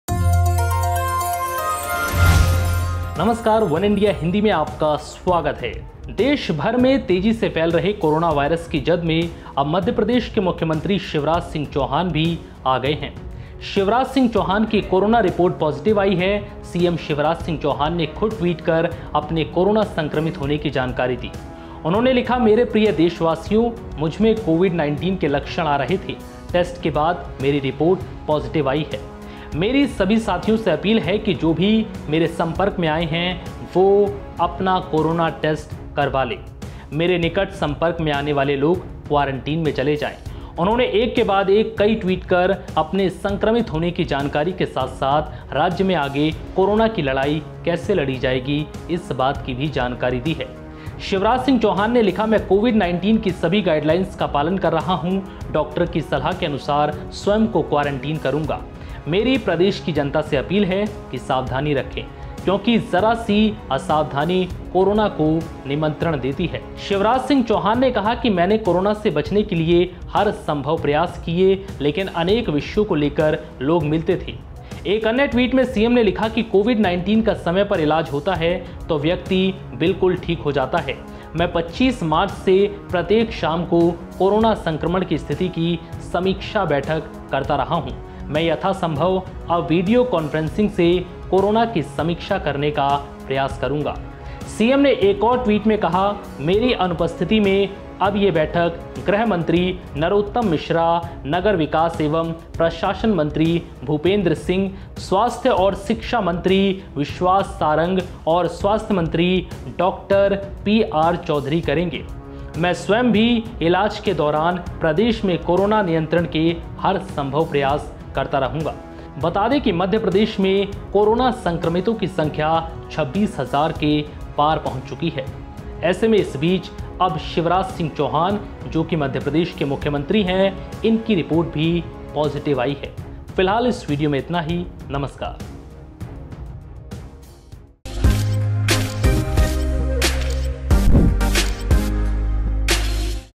Shivraj Singh Chouhan को हुआ Corona, खुद ट्वीट कर दी जानकारी |Covid-19 | वनइंडिया हिंदी
Madhya Pradesh Chief Minister Shivraj Singh Chouhan on Saturday said he has tested positive for Coronavirus. The Madhya Pradesh CM wrote on Twitter that he had symptoms of Coronavirus and now his test result has come positive.Watch video,

देशभर में तेजी से फैले रहे कोरोना वायरस की जद में अब मध्य प्रदेश के मुख्यमंत्री शिवराज सिंह चौहान भी आ गए हैं.  शिवराज सिंह की कोरोना रिपोर्ट पॉजिटिव आई है. सीएम शिवराज सिंह चौहान ने खुद ट्वीट कर अपने कोरोना संक्रमित होने की जानकारी दी है. पूरी जानकारी के लिए ये वीडियो देखें